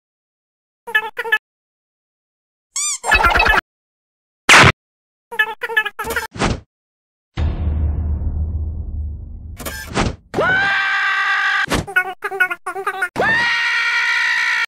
{$hitpost} - A Space Adventure - Short made by @rushedadventurefan4380 - {Stick Nodes}
Something my friend made based on a dream I had that I thought was funny so I uploaded it

Waffles